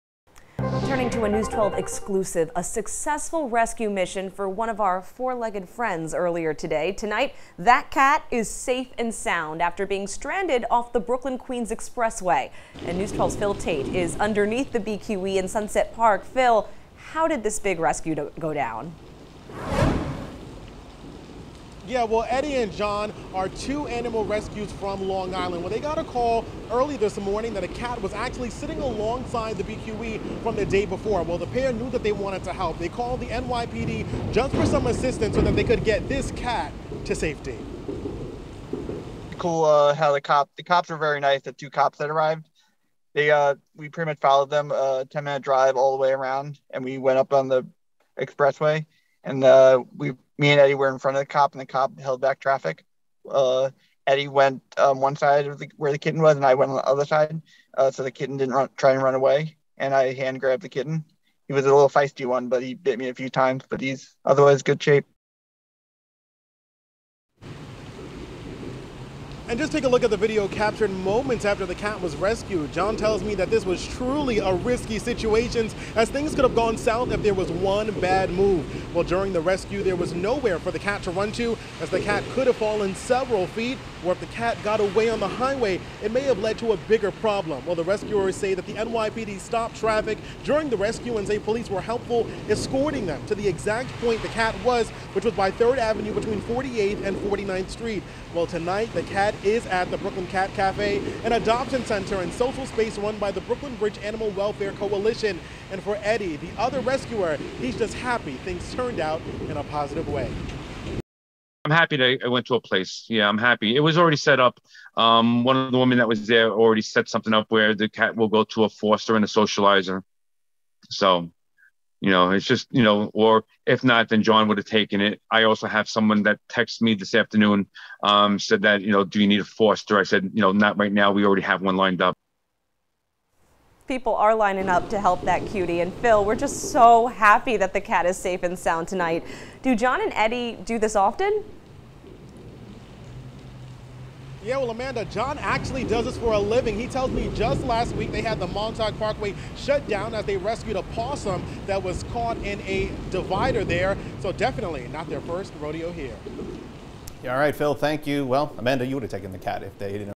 Kitten rescued on the Brooklyn-Queens Expressway, with help from NYPD
Eddie and John are two cat rescuers from Long Island, who received a call Wednesday morning about a cat sitting right along the Brooklyn-Queens Expressway from Tuesday night.